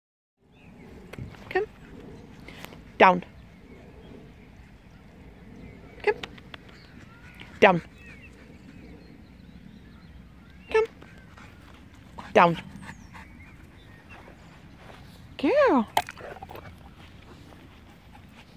Emergency stop.
Trix doing emergency stops.